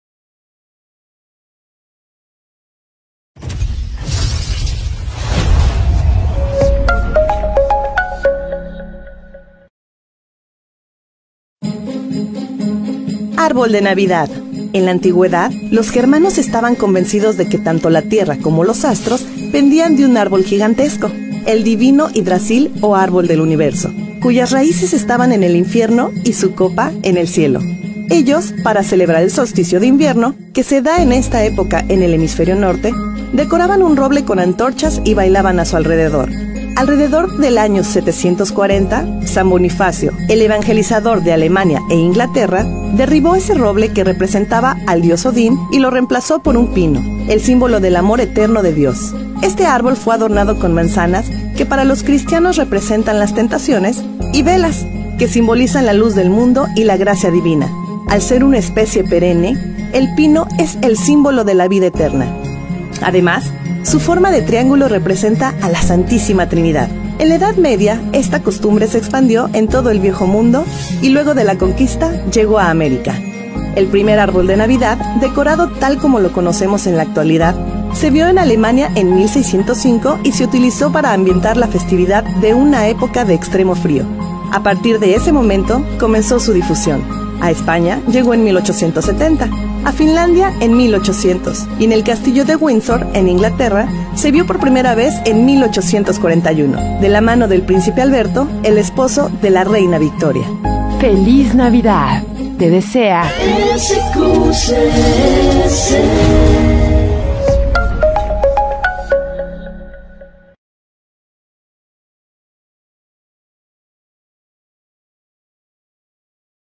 EL ÁRBOL DE NAVIDAD
Es posible que el primer árbol navideño, como se conoce en la actualidad, surgiera en Alemania, donde se implantó por primera vez en 1605 para ambientar el frío de la Navidad, comenzando así su difusión. A Finlandia llegó en 1800, mientras que a Inglaterra lo hizo en 1829, y en el Castillo de Windsor se vio por primera vez en 1841, de la mano del príncipe Alberto, esposo de la reina Victoria.